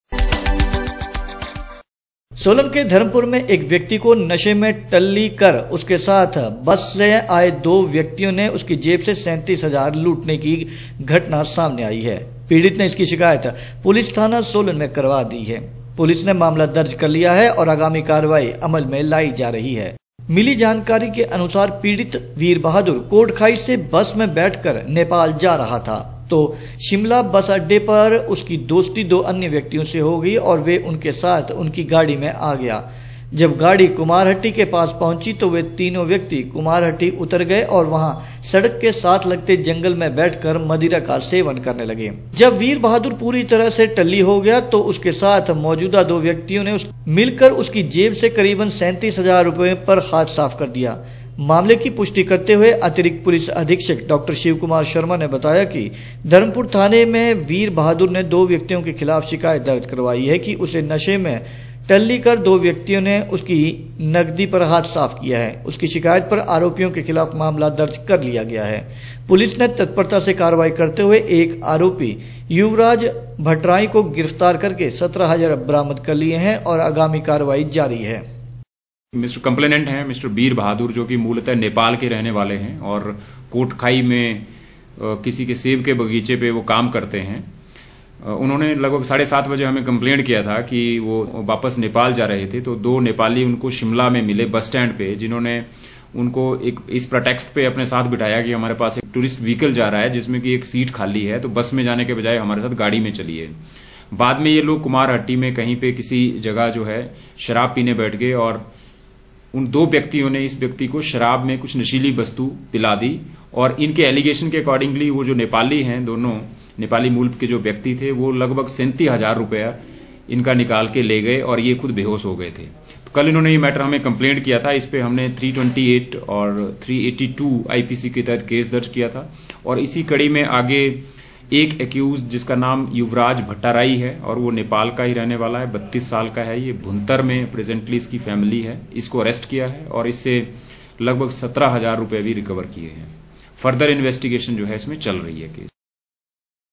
सोलन के धर्मपुर में एक व्यक्ति को नशे में टल्ली करउसकी जेब से 37 हज़ार रूपये लूट लिए
सोलन के धर्मपुर में  एक व्यक्ति को नशे में टल्ली कर उसके साथ बस में आए दो व्यक्तियों ने उसकी जेब से 37 हज़ार रूपये लूट लिए | पीड़ित ने  इसकी शिकायत पुलिस थाने सोलन में दर्ज करवा दी है | पुलिस ने मामला दर्ज कर लिया है और आगामी कार्रवाई अमल में लाई जा रही है | मिली जानकारी के अनुसार पीड़ित वीर बहादुर कोटखाई से बस में बैठ कर नेपाल जा रहा था तो शिमला बस अड्डे पर  उसकी दोस्ती दो अन्य व्यक्तियों से हो गई और वह उनके साथ उनकी गाड़ी में आ गया |